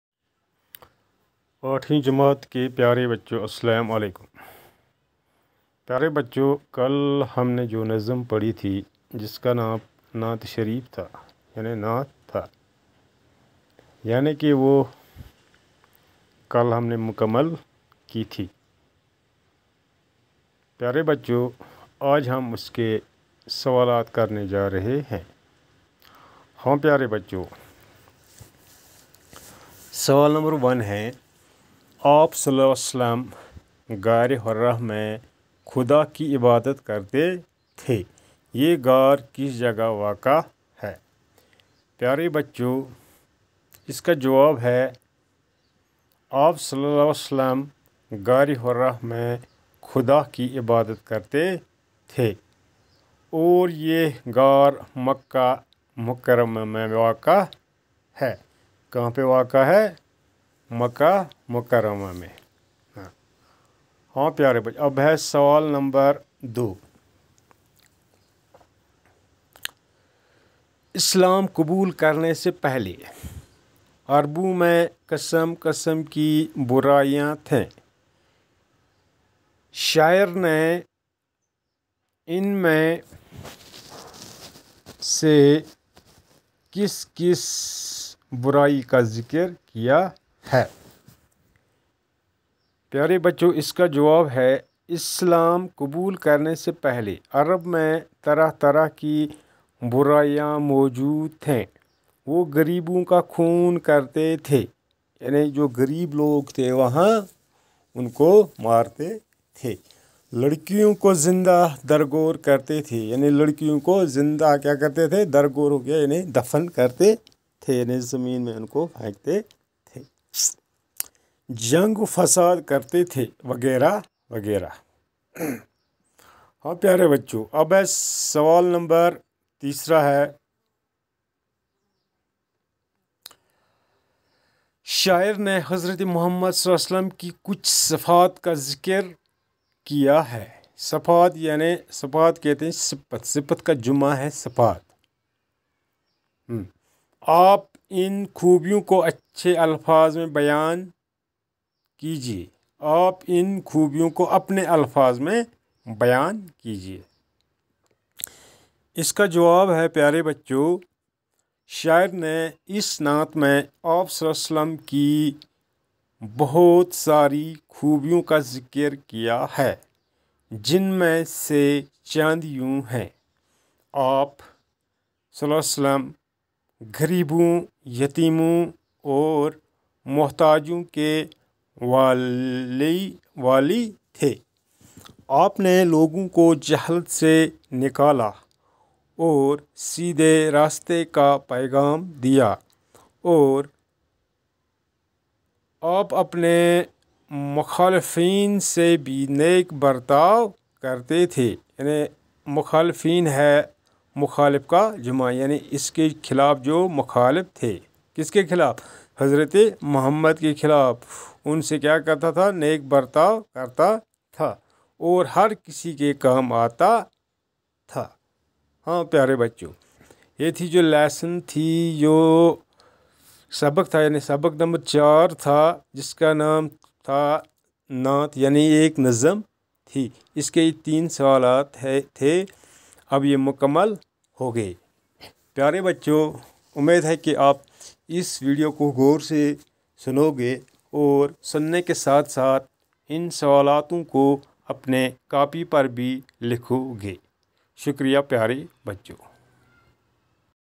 Class-8th, Subject-Urdu, Topic-Exercise of نعت (نظم), سبق نمبر چار
lecturer delivered by Mushtaq Ahmad Mir Teacher UPS Shalibatoo Manigah